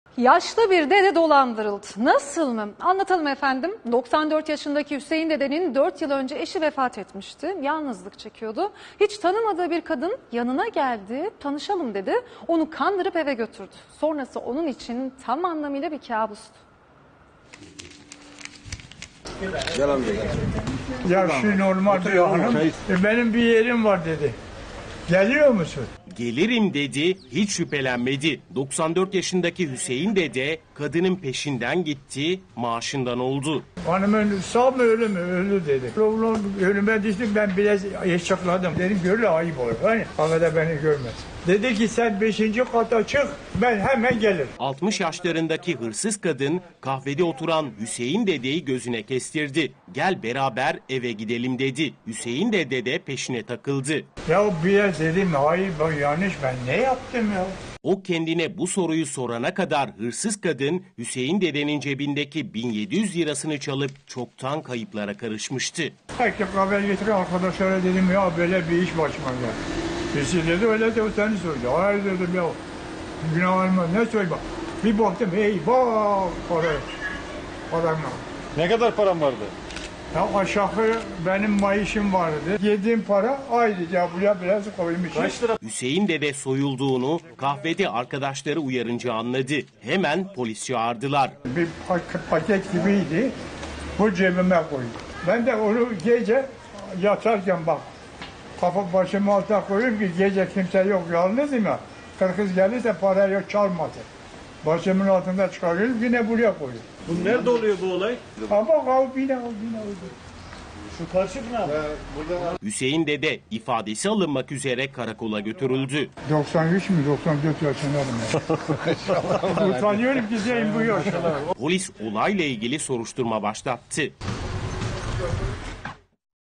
Hüseyin Dede'ye "Tanışalım" tuzağı!
94 YAŞINDAKİ HÜSEYİN DEDENİN 4 YIL ÖNCE EŞİ VEFAT ETMİŞTİ, YALNIZLIK ÇEKİYORDU... HİÇ TANIMADIĞI BİR KADIN YANINA GELDİ,,, TANIŞALIM DEDİ, ONU KANDIRIP EVE GÖTÜRDÜ... SONRASI ONUN İÇİN TAM ANLAMIYLA BİR KABUSTU.